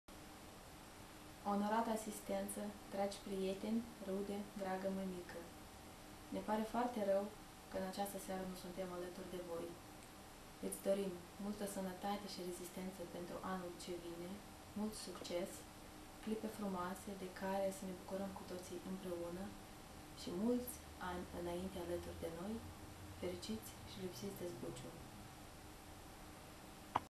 betty
Felicitari de ziua de nastere a Dnei V.Pavlicenco din parte lui Bety, Alex si familia acestuia.